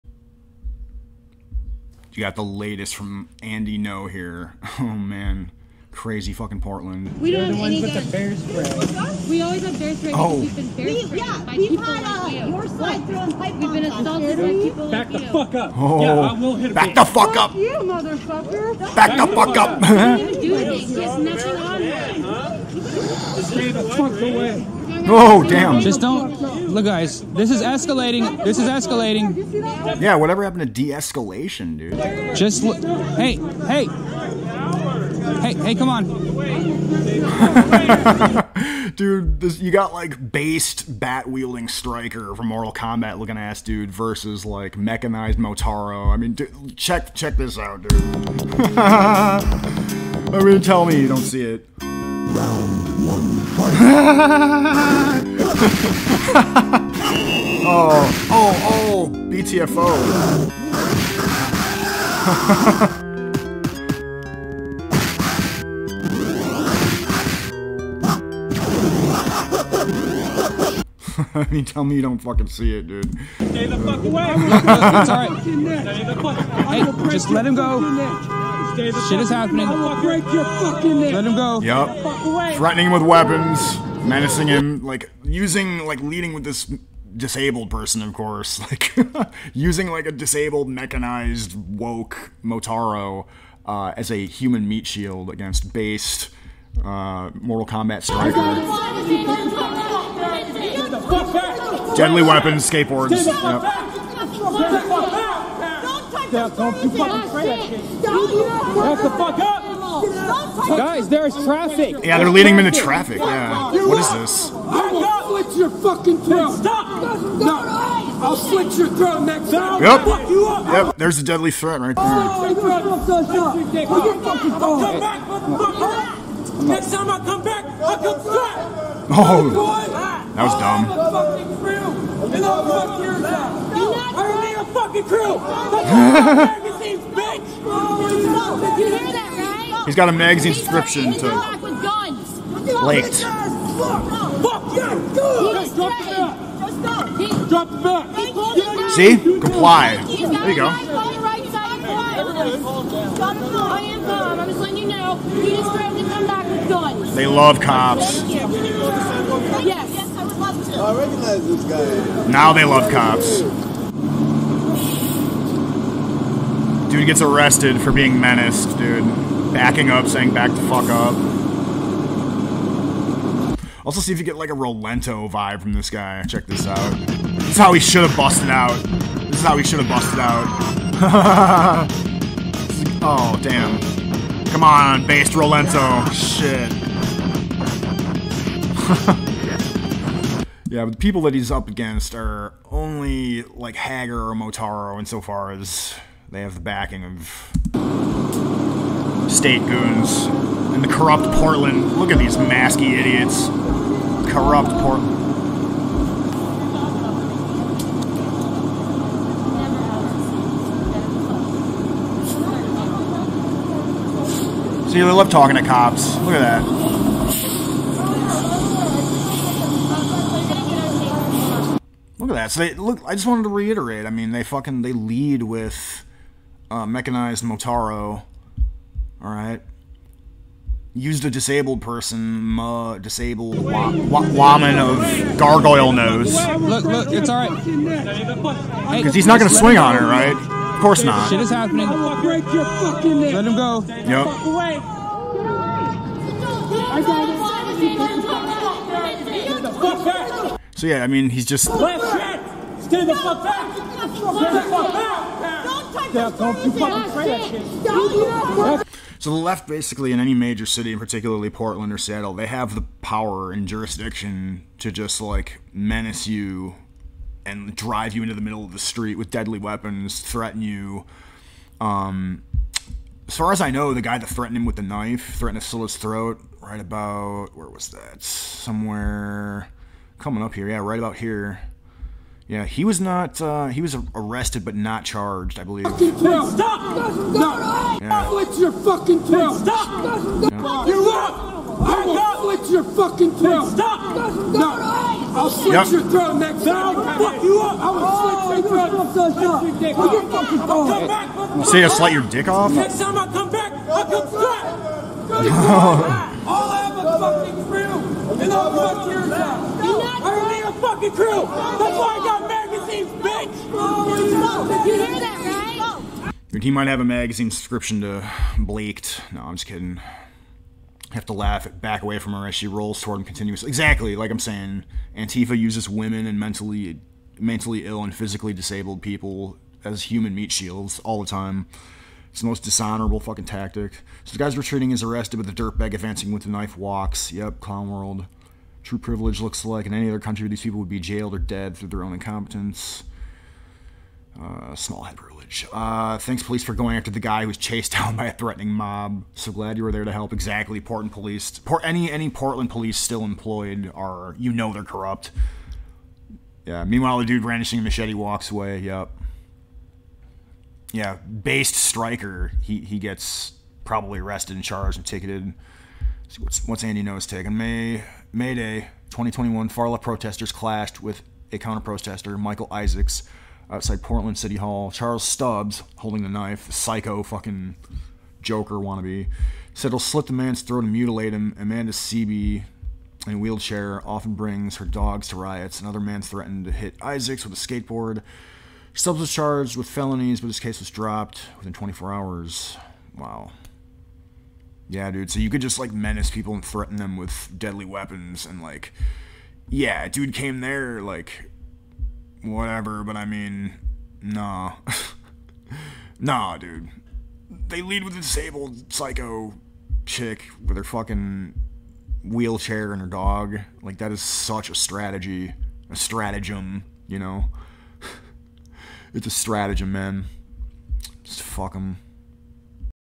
Based Stryker Vs. Mecha Motaro DERANGED Portland Psychosis
SUPPORT ME & FOLLOW ME ON NU TECH IF YOU ARE SICK OF YOUTUBE BS
Breaking down a clip I saw from Andy Ngo's channel showing Based Stryker outta Mortal Kombat (Meets Rolento from Final Fight/Street Fighter Alpha) VS. Woke Mecha Motaro in the hollowed out decayed cracked out husk that is Portland.  Watch these pussy ass cops arrest this dude after these mutants threaten him with deadly weapons, and chase him into the street.  Also notice how leftists will always lead with a woman, or disabled meat shield of some sort.  This is a scummy deliberate guard dropping tactic, obviously.  Also no charges for insaneo knife threatening, of course.  Ghey.